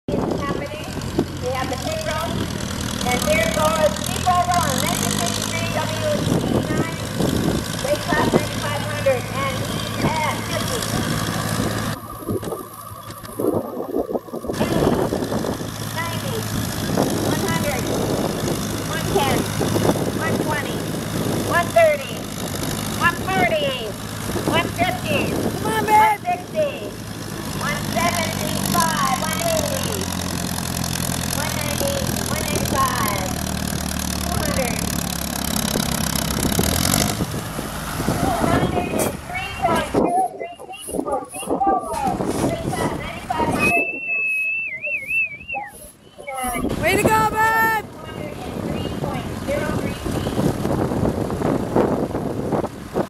Tractor Pull--Driving a 1953 McCormick WD-9 in the 9,500# Weight Class*1,900 Pounds Light.
Here is Steve in his second pull of the day. He pulled 203 feet and came in second place during  Blast to Brooks Antique Tractor Pull. Steve & I came home with three plaques total!